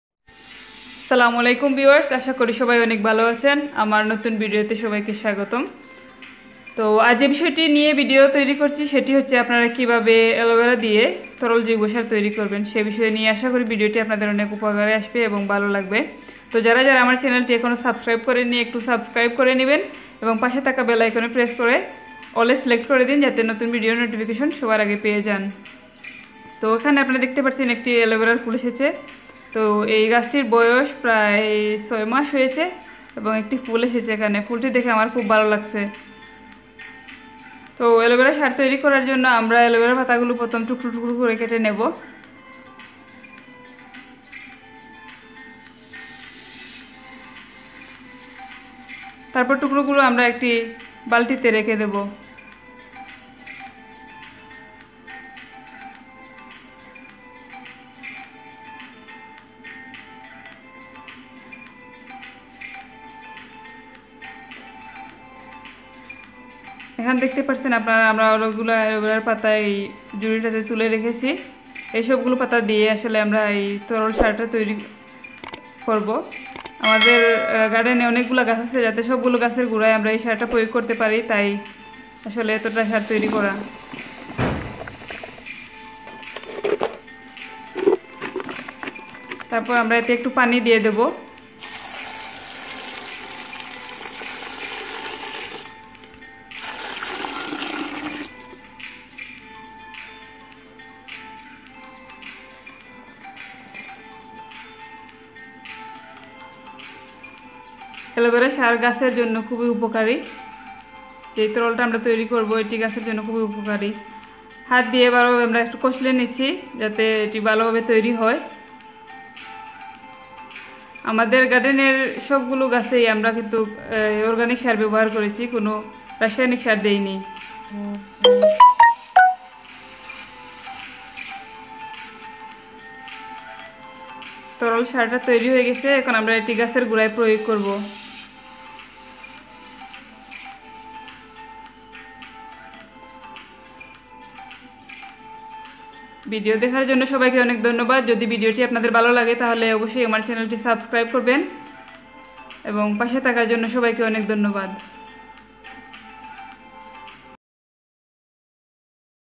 Natural fertilizer from aloe vera|how to make liquid aloevera fertilizer
The video I made today is how you make liquid aloevara fertilizer and how to use it properly and how to make aloevara organic fertilizer.fertilizer for any plant.